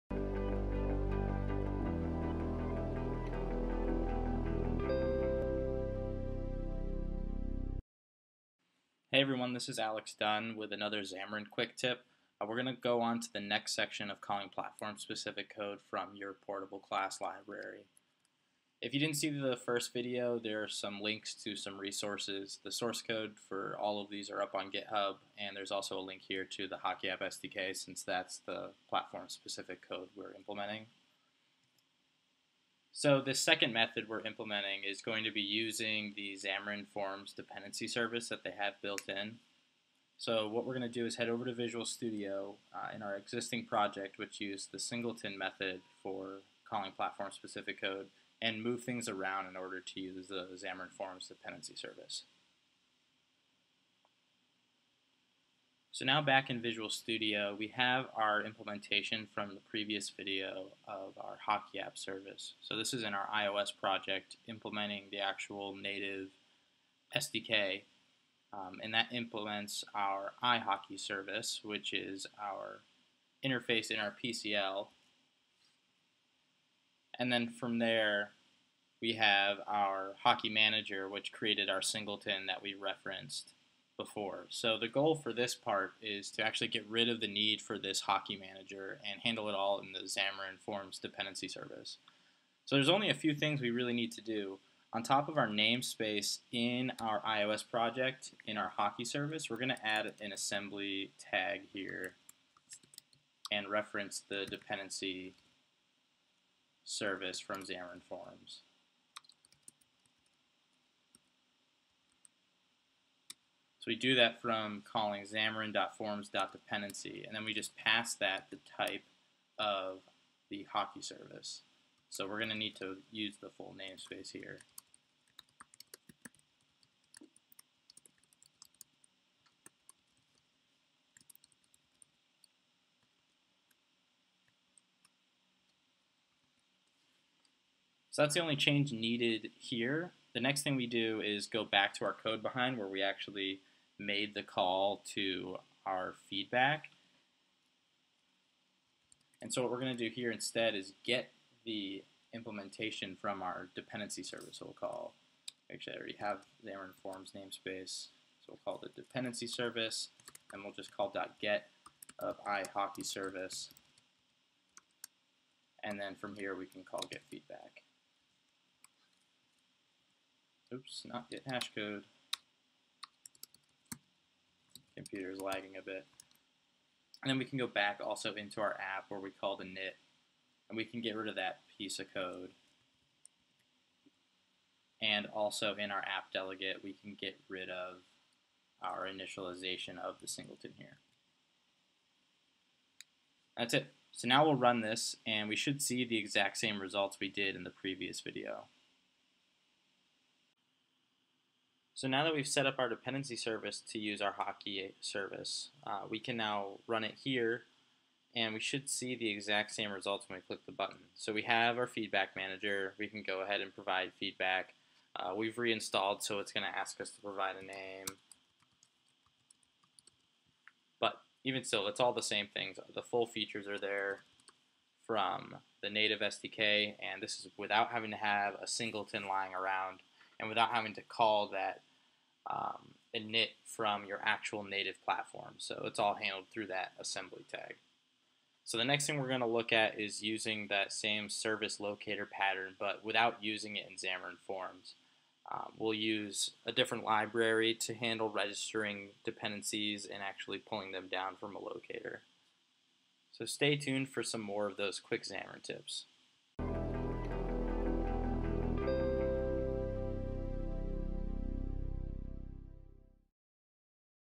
Xamarin.Tips Calling Platform Code from a PCL (Xamarin.Forms DependencyService)
Using the Xamarin.Forms DependencyService class to call platform specific code from shared code in Xamarin. Make calls to the HockeyApp iOS SDK from a Portable Class Library.